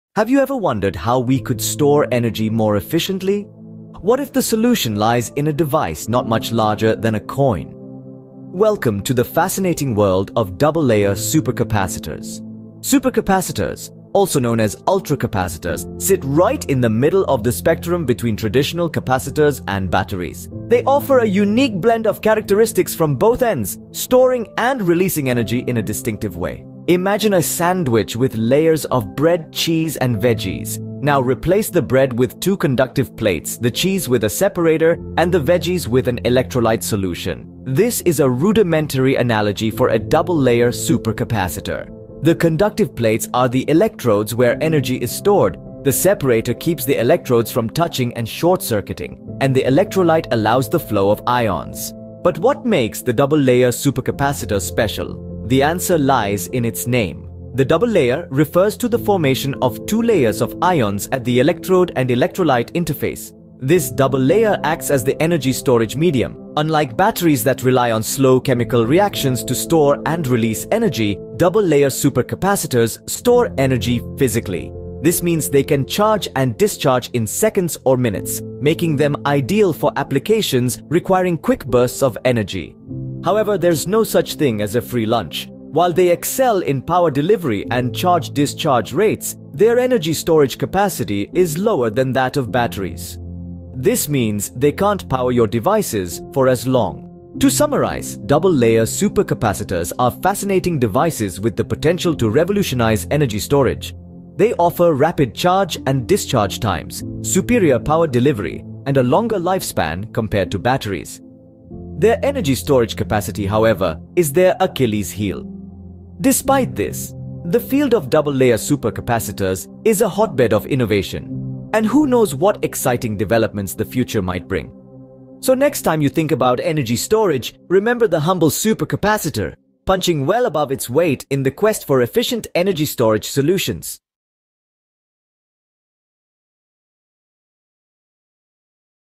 Unveiling the Power of Double Layer Supercapacitors
An experimental AI created/ edited video ( ai.invideo.io) with collaborative input from Professor David S. Soriano ( University of Pittsburgh-Bradford) in PA., USA.